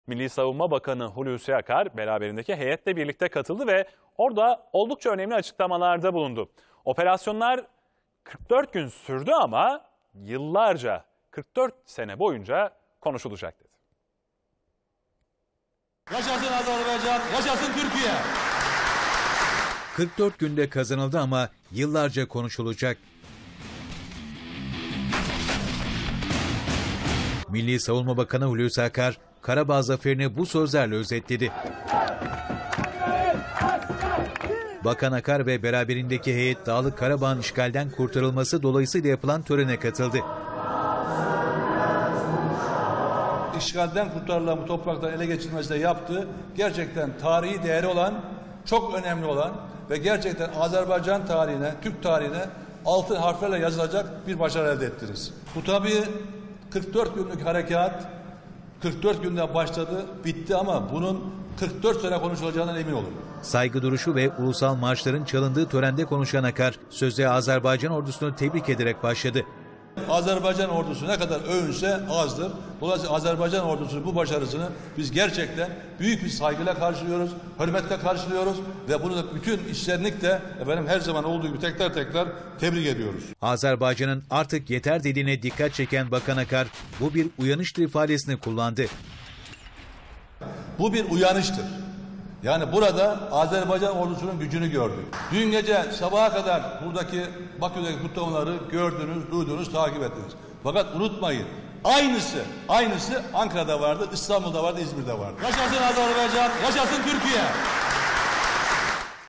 "44 Günde Kazanıldı 44 Sene Konuşulacak!" / A Haber
Milli Savunma Bakanı Hulusi Akar, Bakü’de Azerbaycan Savunma Bakanlığı’nı ziyaret etti. Akar, ‘’Azerbaycan Silahlı Kuvvetleri’nin öz topraklarını işgalden kurtarmak için gerçekleştirdiği 44 günlük harekat sırasında gösterdiği kahramanlık ve cesaret takdire şayandır. Harekat 44 günde bitti ama bunun 44 sene boyunca konuşulacağından emin olun’’ dedi.